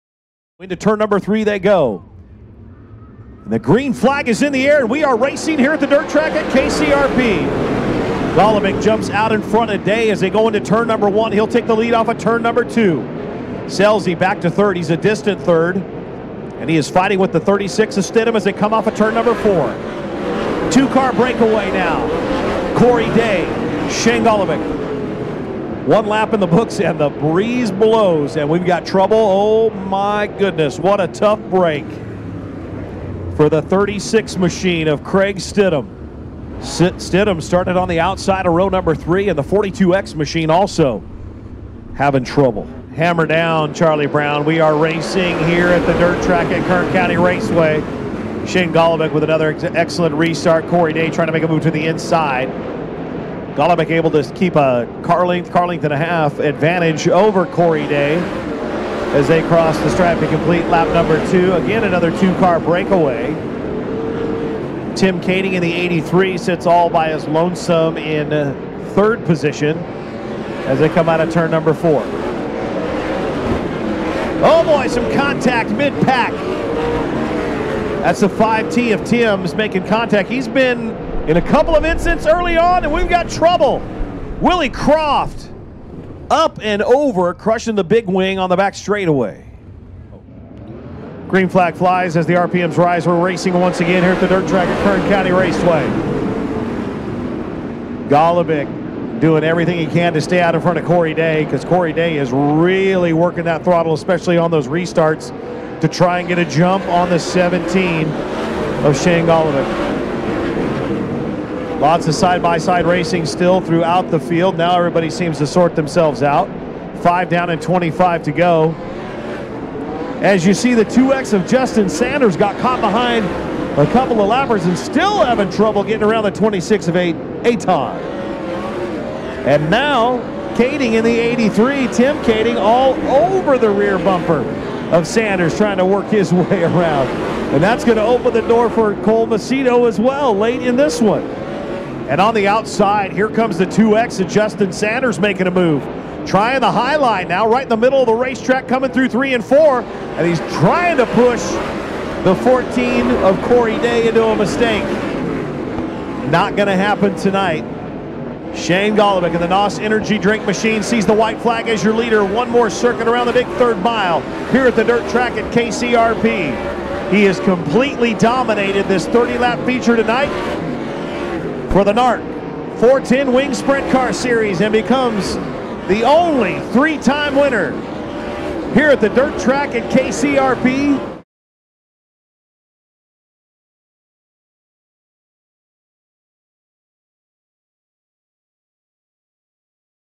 NARC SPRINT CARS - DIRT TRACK @ KCRP - OCTOBER 29, 2022
The NARC Fujitsu General Sprint Car Series made their only appearance of the season at the Dirt Track at Kern County Raceway Park on October 29th.  Action was fast and furious, but one team dominated.  ENJOY!